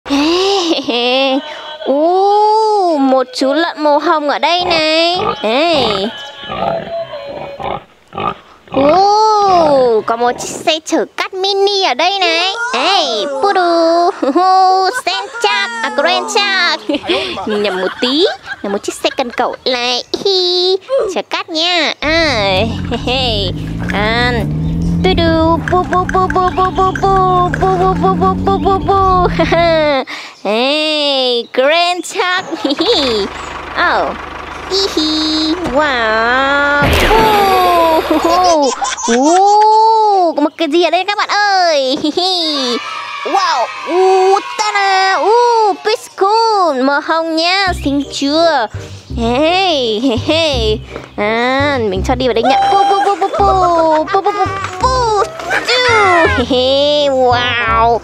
Mini Toy World | mini sand truck - mini crane - mini bus - tractor - tractor truck
Let's explore the toys in this video. There will be many interesting things for you. Watch the video and and help the little ones Toys Review & learning name & sounds and color discrimination
Toys Review & how to learning any things name & sounds

Note ÷
This video is only entertainment purposes.